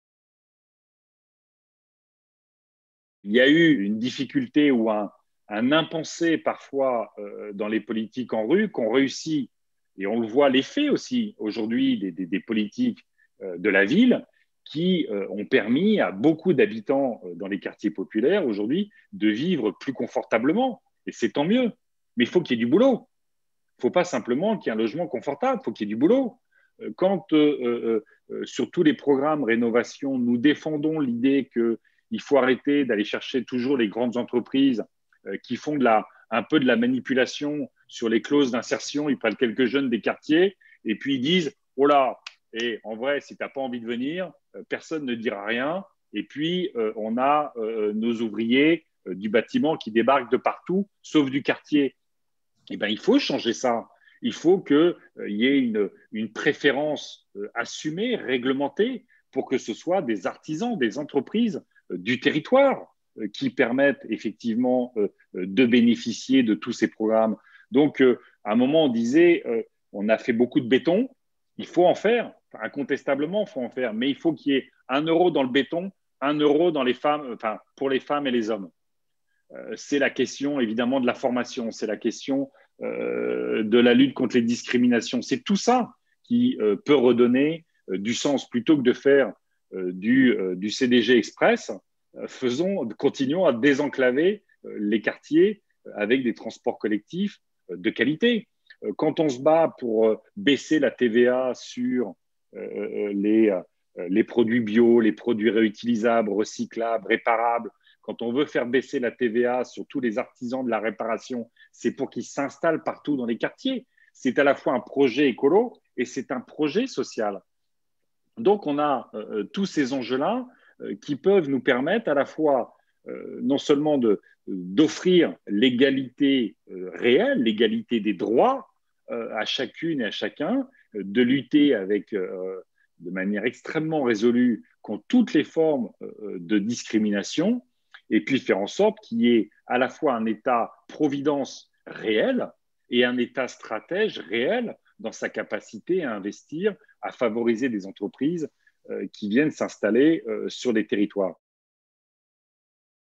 Réponses aux militants - #5 Quelles politiques pour nos quartiers populaires?
Yannick Jadot répondait aux questions de militants de Seine-Saint-Denis et du Val-de-Marne à l'occasion d'un webinaire le mardi 20 avril 2021. Il revient ici sur les mesures sociales clés à adopter pour le retour d'un réel État-Providence stratège.